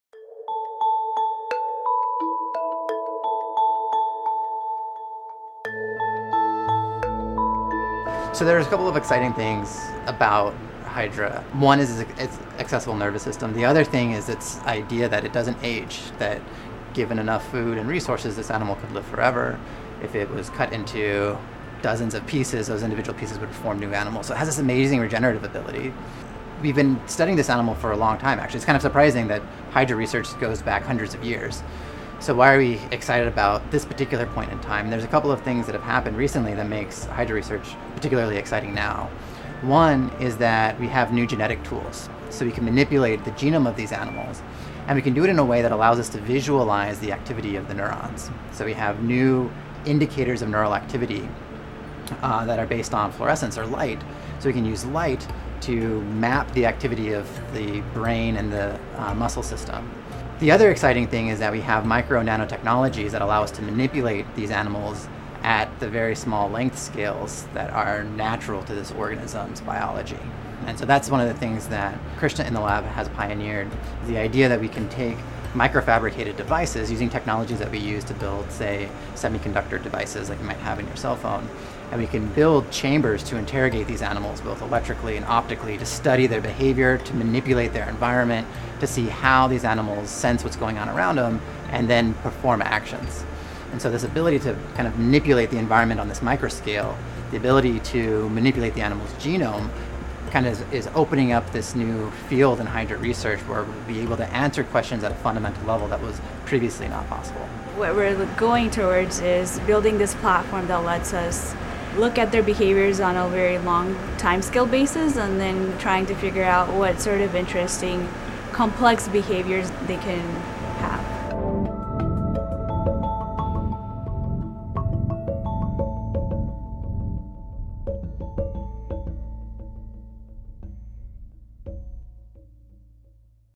Squishy hydra's simple circuits ready for their close-up
Just because an animal is soft and squishy doesn't mean it isn't tough. Experiments at Rice University show the humble hydra is a good example.

 The hydra doesn't appear to age – and apparently never dies of old age. If you cut one in two, you get hydrae. And each one can eat animals twice its size.

These beasties are survivors, and that makes them worthy of study, according to Rice electrical and computer engineer Jacob Robinson.

Robinson and his team have developed methods to corral the tiny, squid-like hydrae and perform the first comprehensive characterization of relationships between neural activity and muscle movements in these creatures. Their results appear in the Royal Society of Chemistry journal Lab on a Chip.